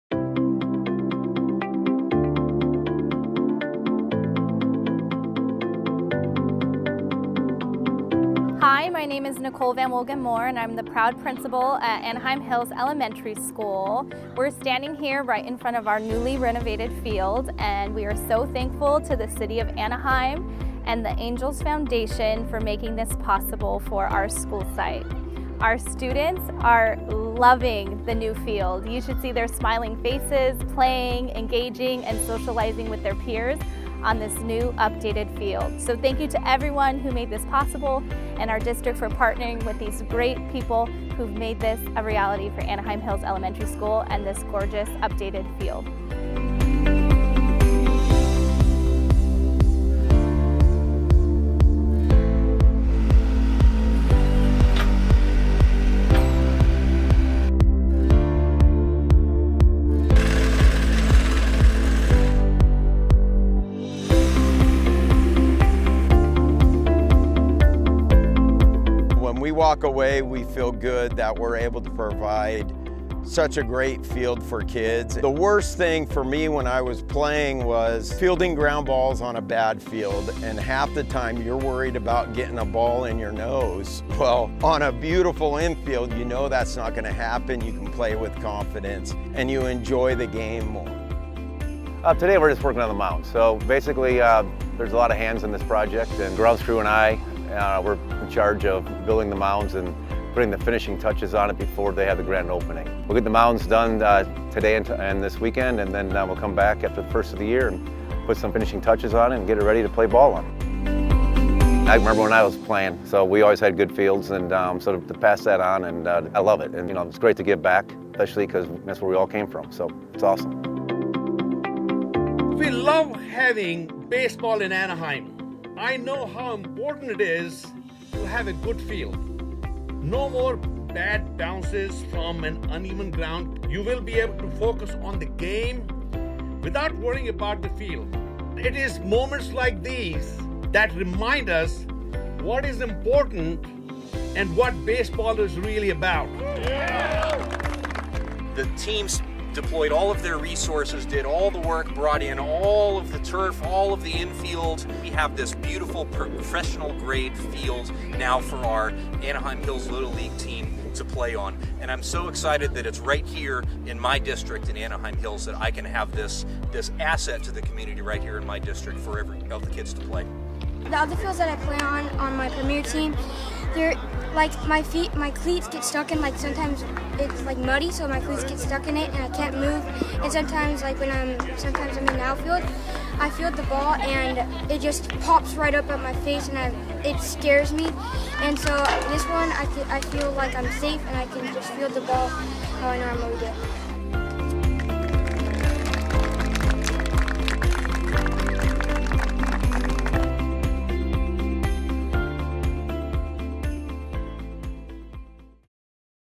Anaheim Hills Elementary and Little League Fields Re-open as New "Big A Ballpark"
The Angels Baseball Foundation, with support from the City of Anaheim and other donors, refurbished two fields used by Anaheim Hills Elementary School and Anaheim Hills Little League.  The Angels worked closely with Magical Builders, a construction management non-profit, to provide a safe and enjoyable place for kids to play the game of baseball.